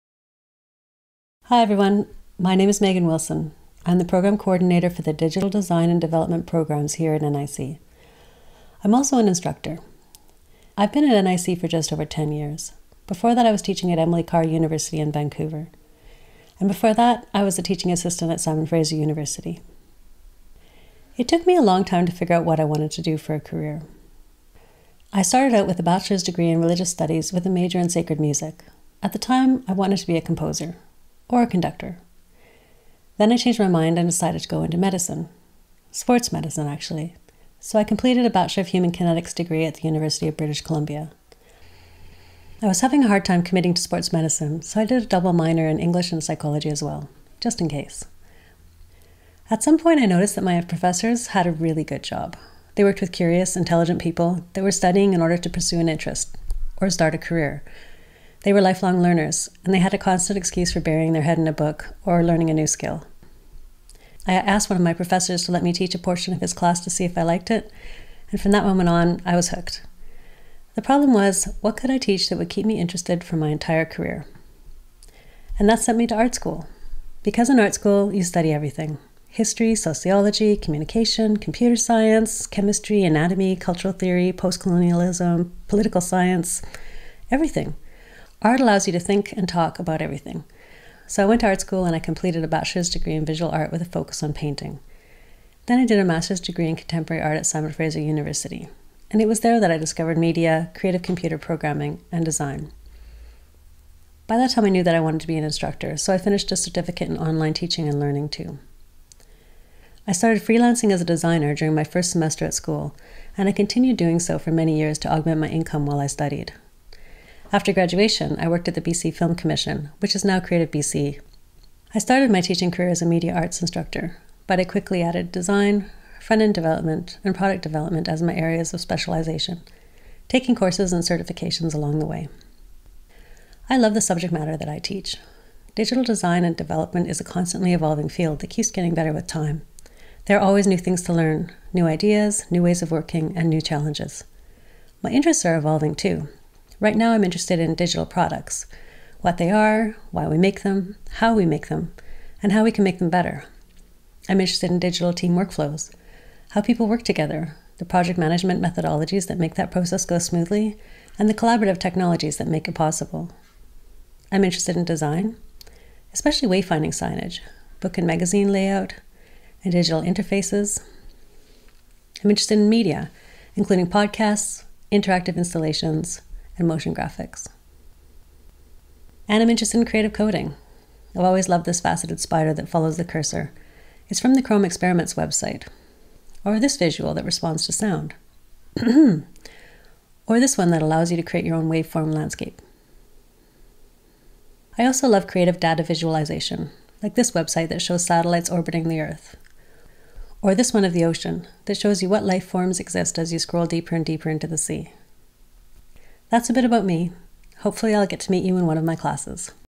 Megan Wilson
Faculty profile for Megan Wilson, an instructor in the DIGITAL Design + Development programs at North Island College.